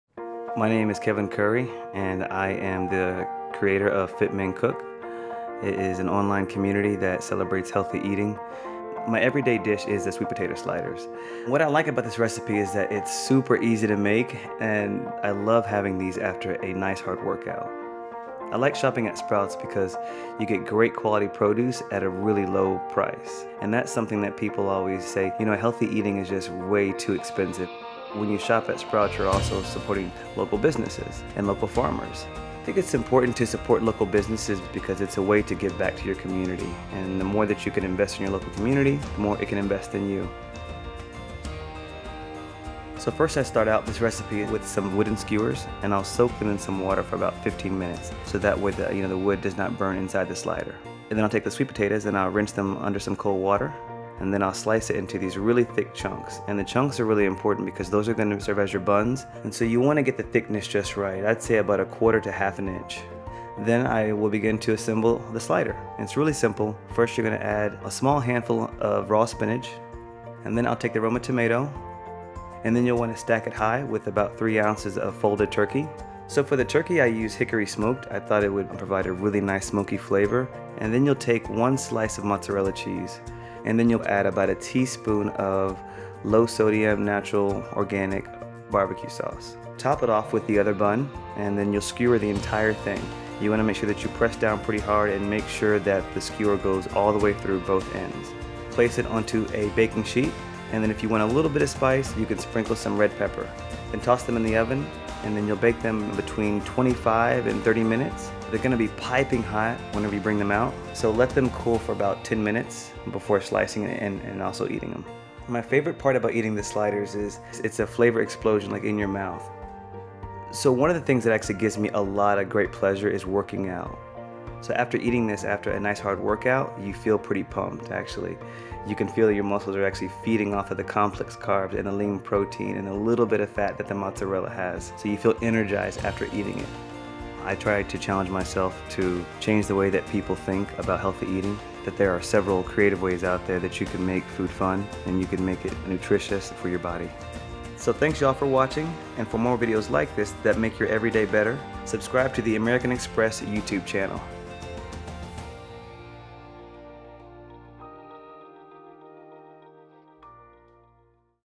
Sweet Potato Sliders w/ Kevin Curry | My EveryDay Dish
Kevin Curry, YouTube personality behind Fit Men Cook, celebrates healthy and easy eating cooking tips as well as the importance of buying local. Kevin's EveryDay Dish is Sweet Potato Sliders, the perfect post-workout meal.

Ingredients for 2 sliders:

Wooden kabob skewers
200g (4 x 50g) sweet potato cut into thick pieces
6oz sliced turkey (I prefer hickory smoked)
1 slice mozzarella cheese
Small handful of spinach
2 Roma tomato slices
2 tsp low-sodium BBQ sauce
Red pepper flakes (optional)

Instructions:

1) Set oven to 375F. Soak wooden kabob skewers in water for about 15 minutes.
2) Slice a sweet potato into 4 thick pieces to make the “buns” of the sliders, about _ to _ an inch thick.
3) Cut the slice of mozzarella cheese into 4 pieces.
4) On one sweet potato slice, add spinach, 1 tomato slice, 3oz turkey meat, 1 mozzarella cheese pieces, 1 tsp BBQ sauce.
5) Top with the other sweet potato slice and push a wooden kabob skewer all the way through the entire slider so that it pierces the sweet potato “bun” below.
6) Repeat for the other slider. Sprinkle with red pepper flakes is desired.
7) Place them on a baking sheet and bake for 25-30 minutes at 375F, or until the sweet potato is cooked to desired firmness/softness. (I prefer them to be firm so that you can hold it like a traditional slider.)
8) Allow the sliders to cool for about 10 minutes before slicing and serving.